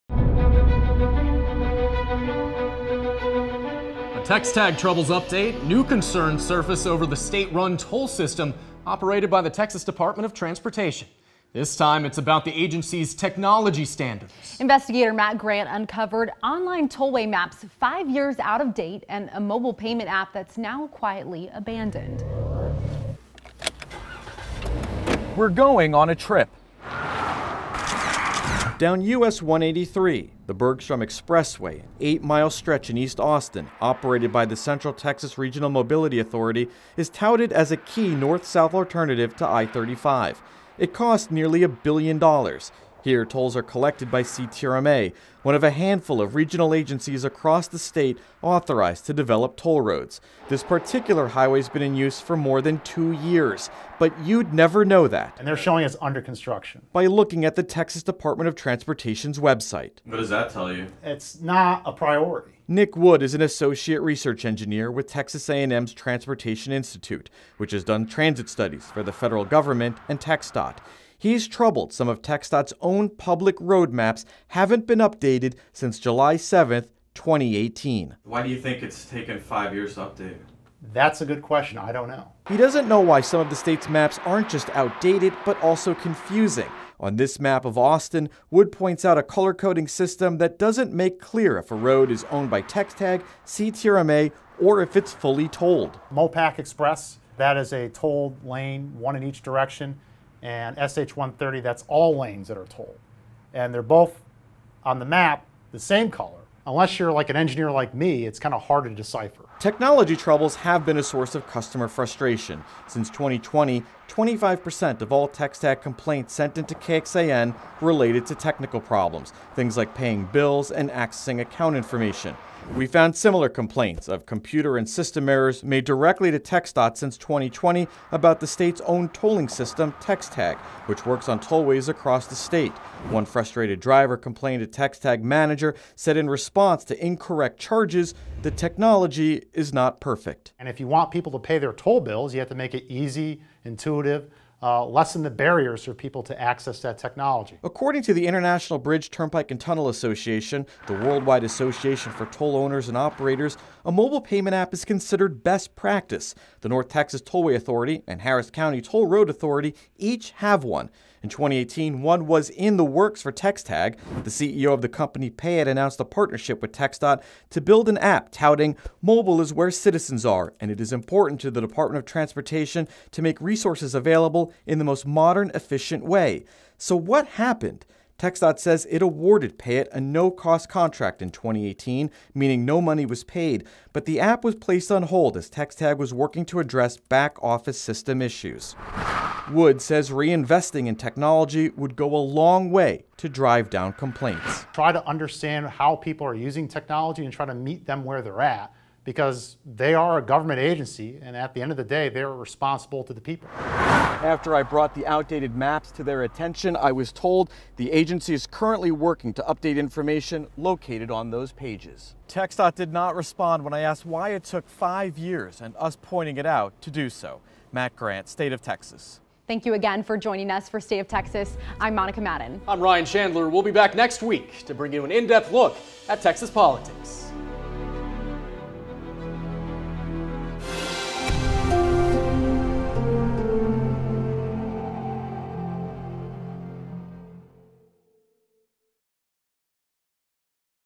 State of Texas: Abandoned app, outdated maps, investigation finds TxTag tech concerns
TxTag Technology Trouble – Our investigation finds maps five years out of date and a much-needed mobile payment app abandoned.  What happened after our investigators started asking questions.